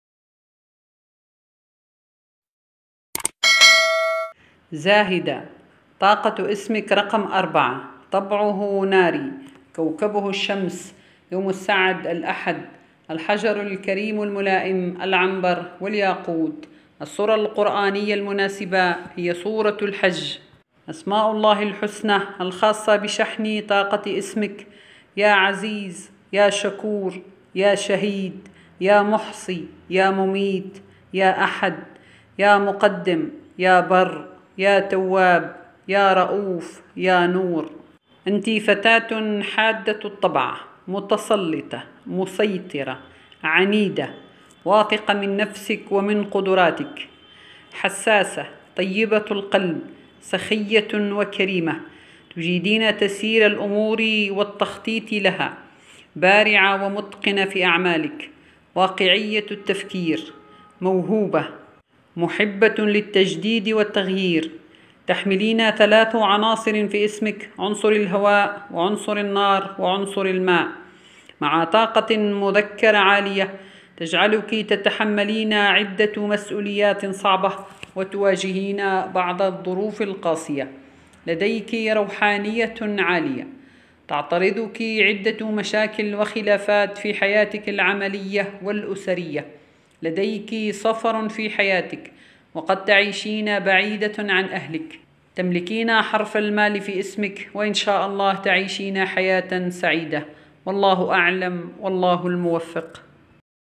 تحليل إسم # زاهدة
فيديو يدرس ويحلل إسم زاهدة